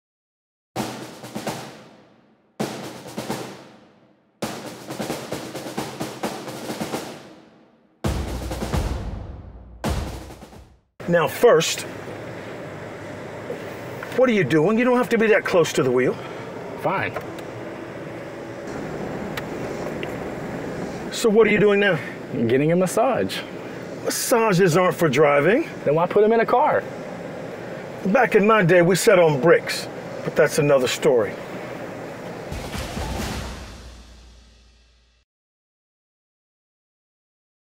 Ed Driver: Massage Seats
📢 Welcome back Trainee's!! This week we learn about massage seats and why they're better than bricks. We hope you enjoy this lesson!